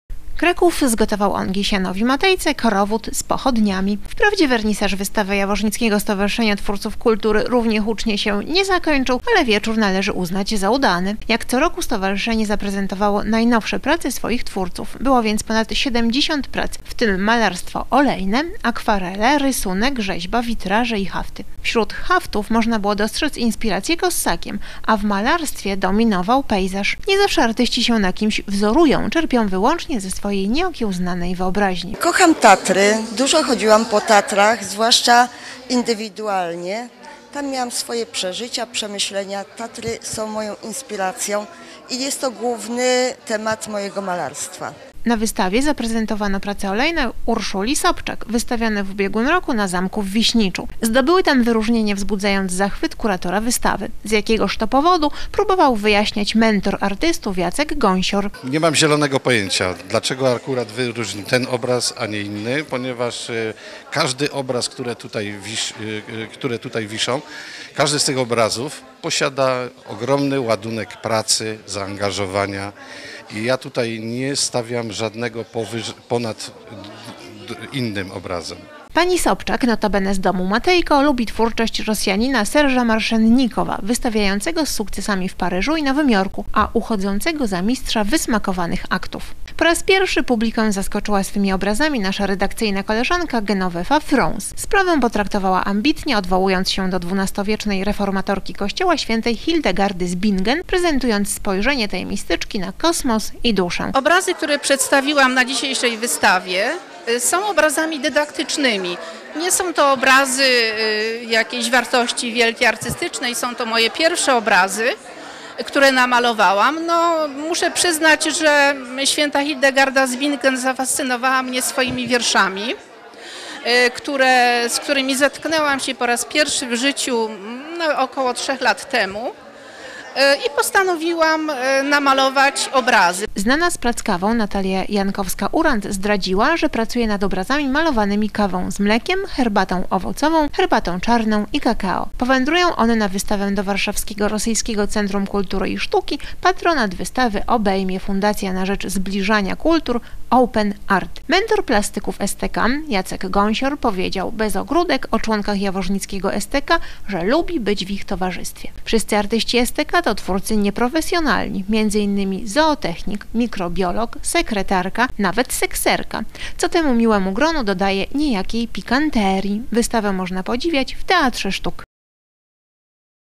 Wernisaż wystawy STK
Kraków zgotował ongiś Janowi Matejce korowód z pochodniami. Wprawdzie wernisaż wystawy

jaworznickiego Stowarzyszenia Twórców Kultury równie hucznie się nie zakończył ale wieczór

należy uznać za udany. Jak co roku stowarzyszenie zaprezentowało najnowsze prace swoich

twórców. Było więc ponad 70 prac w tym malarstwo olejne, akwarele, rysunek, rzeźba, witraże i

hafty.

Wśród haftów można było dostrzec inspiracje Kossakiem, w malarstwie dominował pejzaż. Nie

zawsze artyści się na kimś wzorują, czerpią wyłącznie ze swojej nieokiełznanej wyobraźni.